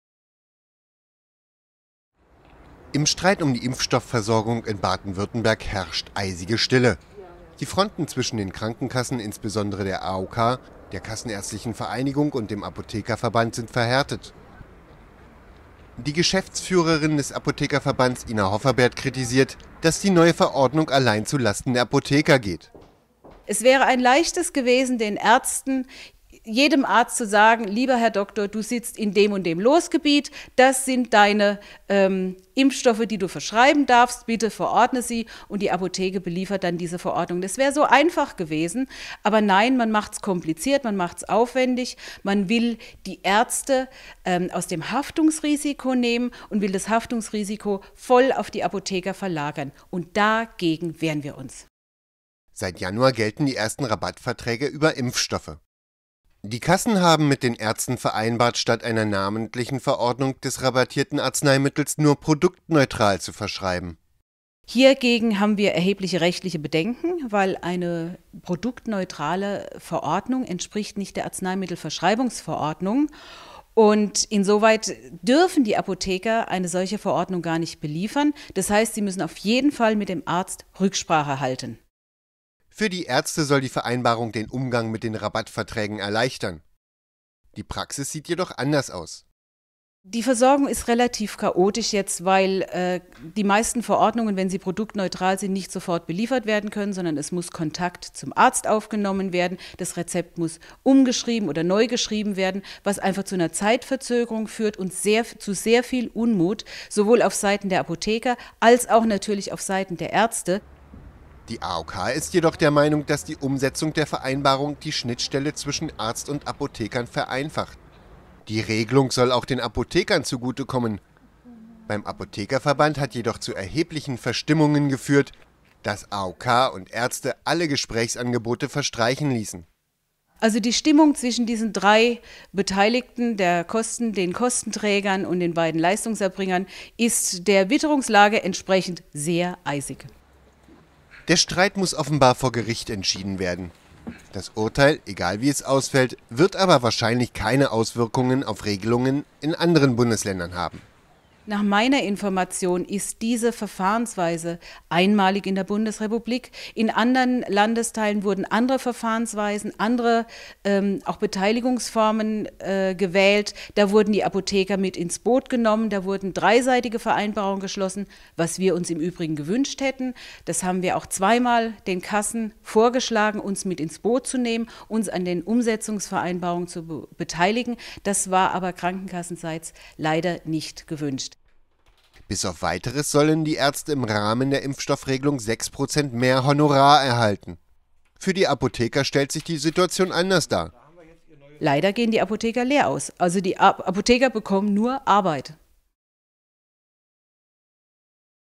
Hofferberth: Apotheker haften nicht für Ärzte
Im Streit um die Impfstoffversorgung in Baden-Württemberg herrscht Eiszeit. Die Fronten zwischen Krankenkassen, Ärzten und dem Landesapothekerverband (LAV) sind verhärtet. AOK und Kassenärztliche Vereinigung (KV) haben alle Gesprächsangebote verstreichen lassen. Der Streit muss offenbar vor Gericht entschieden werden.